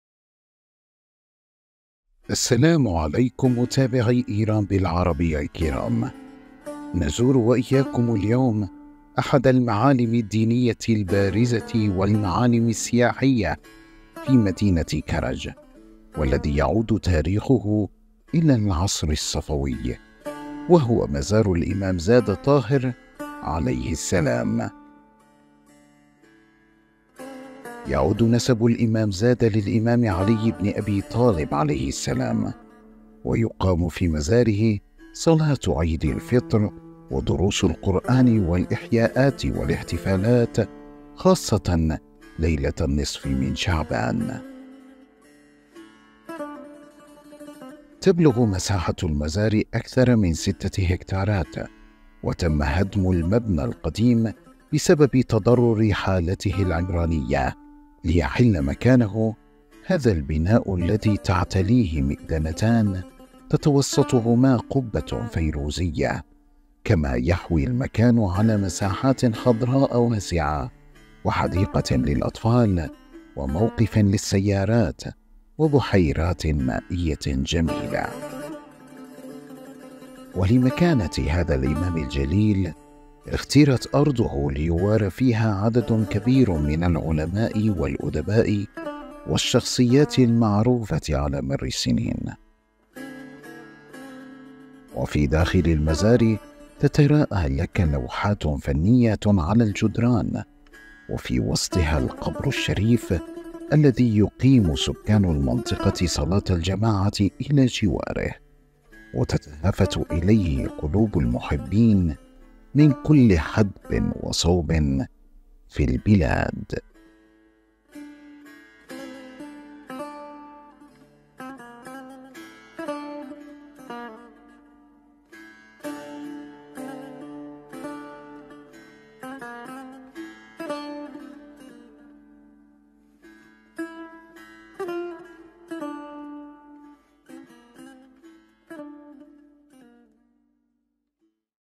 مزار الامامزاده طاهر عليه السلام في كرج
السلام عليكم متابعي إيران بالعربية الكرام

نزور وإياكم اليوم أحد المعالم الدينية البارزة والمعالم السياحية في مدينة كرج والذي يعود تاريخه إلى العصر الصفوي، وهو مزار الإمام زادة طاهر عليه السلام.

يعود نسب الإمام زادة للإمام علي بن أبي طالب عليه السلام، ويقام في مزاره صلاة عيد الفطر ودروس القرآن والإحياءات والاحتفالات خاصة ليلة النصف من شعبان.

تبلغ مساحة المزار أكثر من 6 هكتارات، وتم هدم المبنى القديم بسبب تضرر حالته العمرانية ليحل مكانه هذا البناء